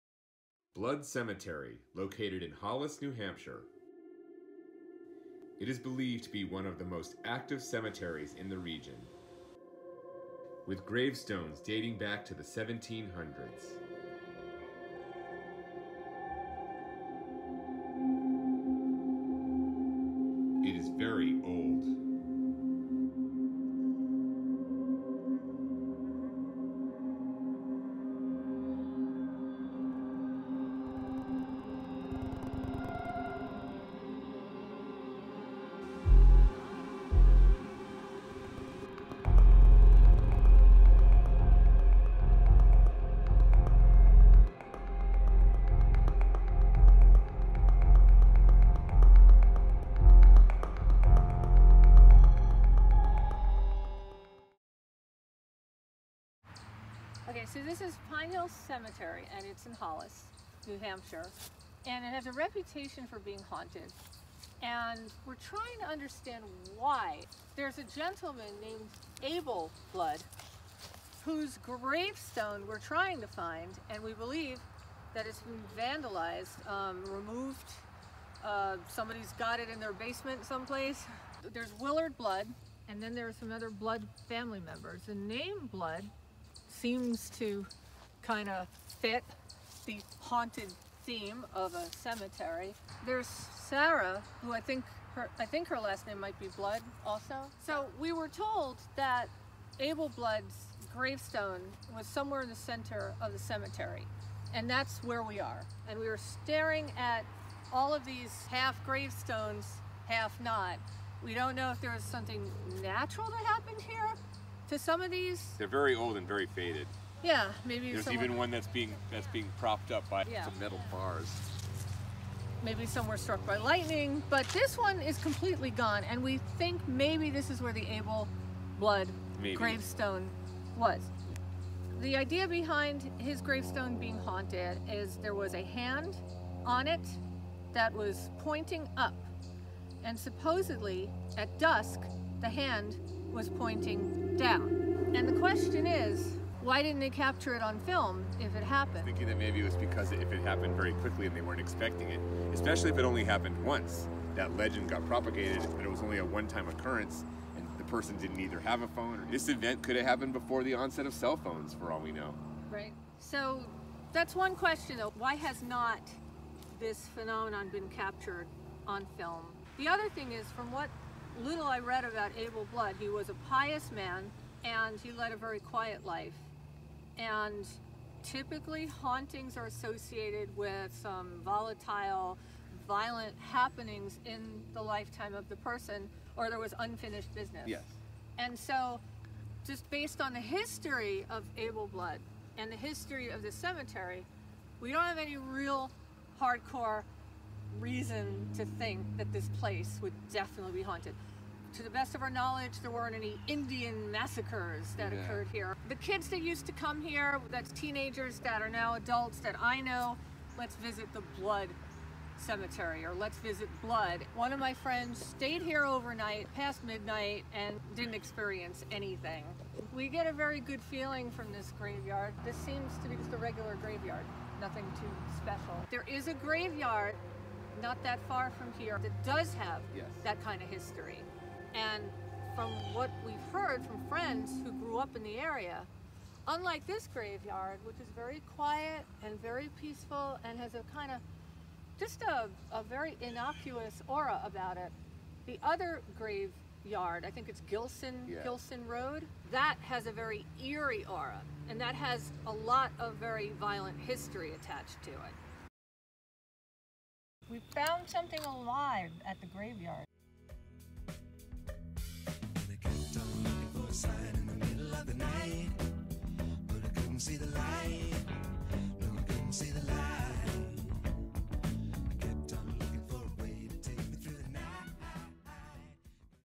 Blood Cemetery (Pine Hill Cemetery, Hollis, NH)
Part 2 of our New England Graveyard series.
See also Part 1, the Old Burying Yard in York, ME
Photography by Emily Yoffe, Ken Yoffe, and Ellen Weisberg, except the photos at 0:14, 0:35, 0:40, and 3:35 which are not original.